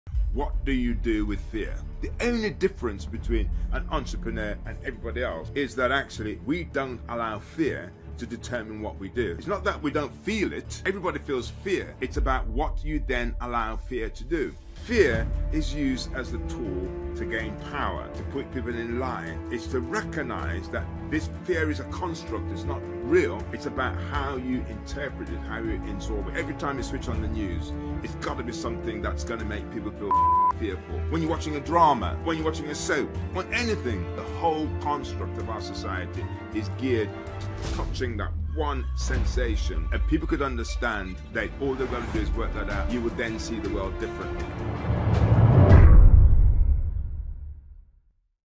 The key to success? Managing Fear
Wilfred Emmanuel Jones - The Black Farmer - has some hard-won advice for success - the answer is in you.